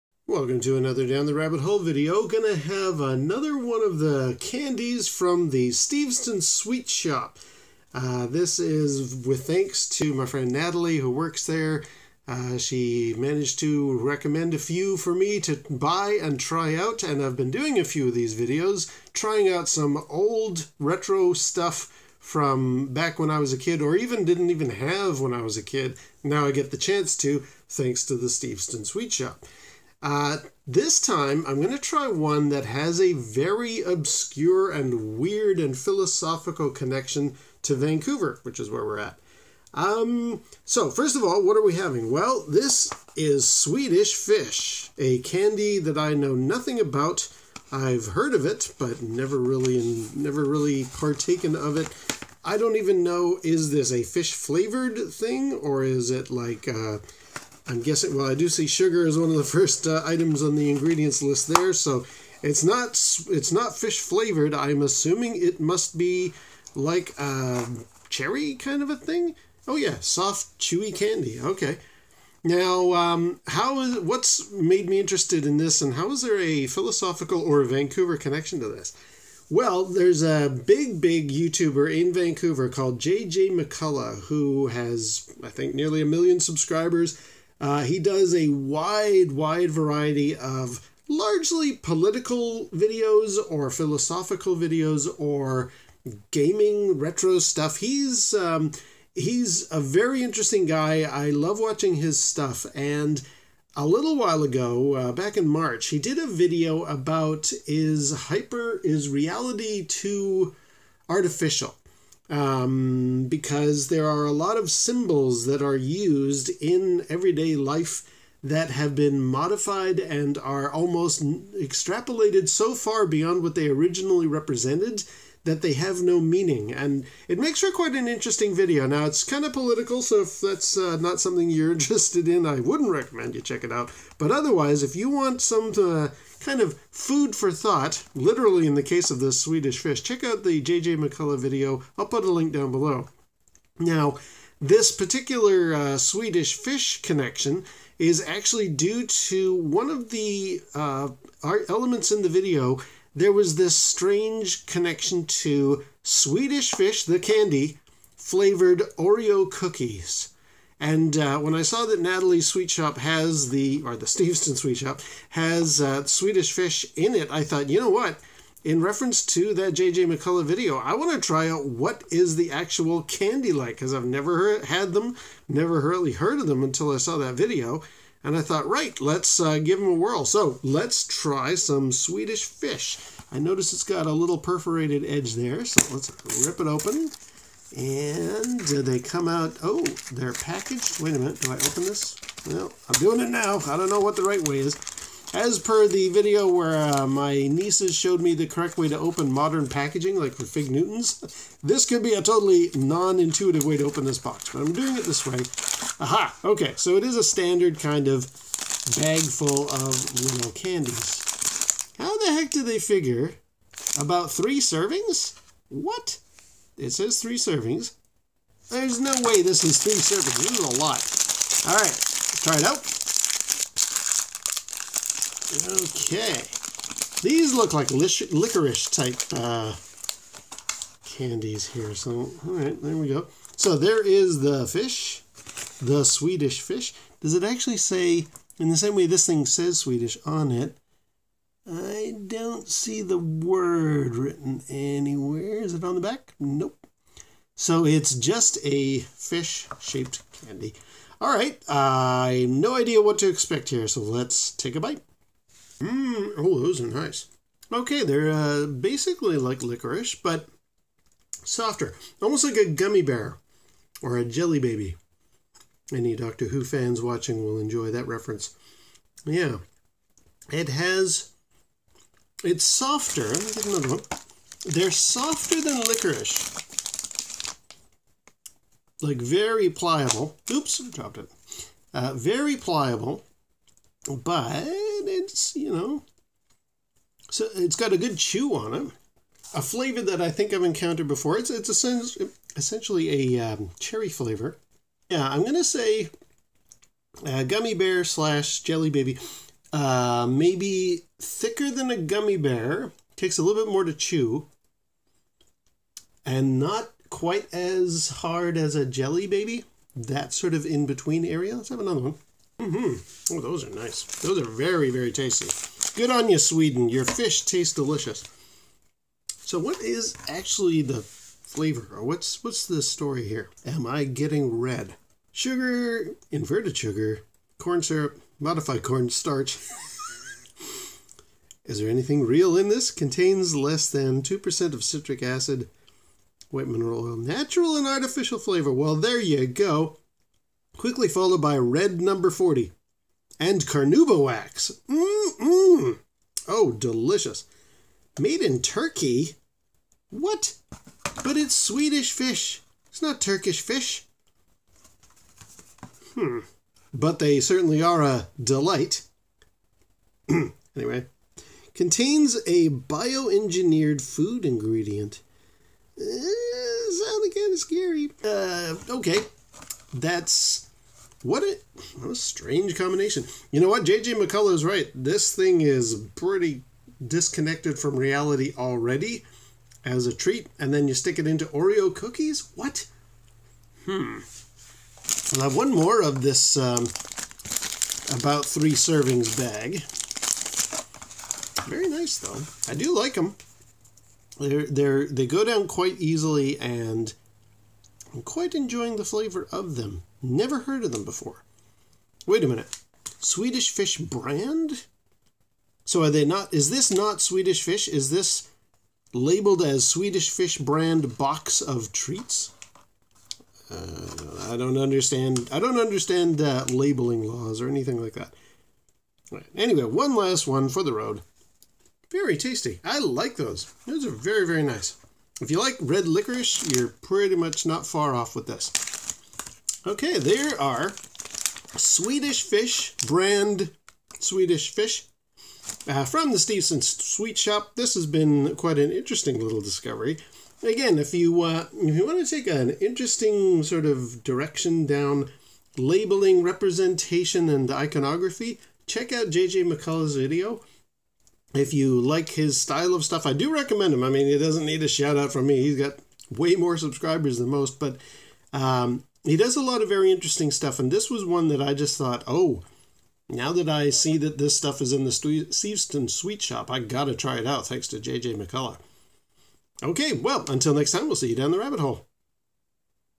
Swedish Fish from the Steveston Sweet Shoppe has more than one Vancouver connection
Going to try Swedish Fish candy from the Steveston Sweet Shoppe because I discovered it has more than one obscure and philosophical Vancouver connection.

Steveston Sweet Shoppe's location:
12031 First Ave 120, Richmond, BC
also, follow them on Instagram

J.J. McCullough's original video